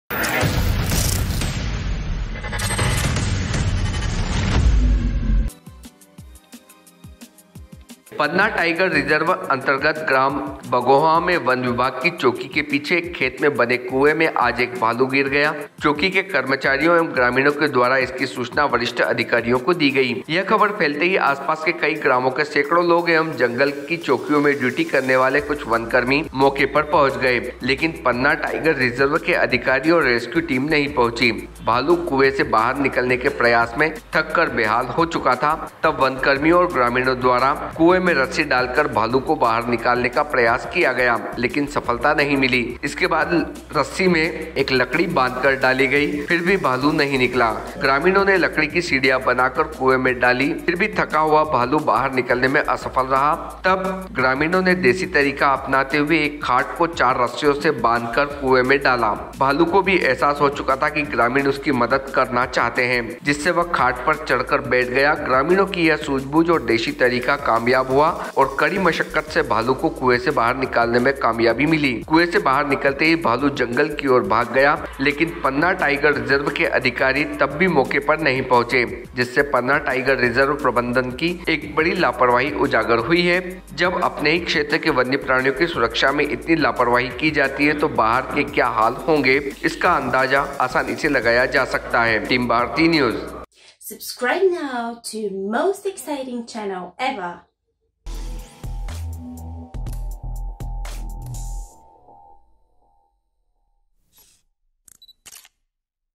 कुएं में गिरा भालू नही पहुंची वन विभाग की टीम,भालू को कुएं से निकालने ग्रामीणों ने अपनाया देशी तरीका
Panna / Madhya Pradesh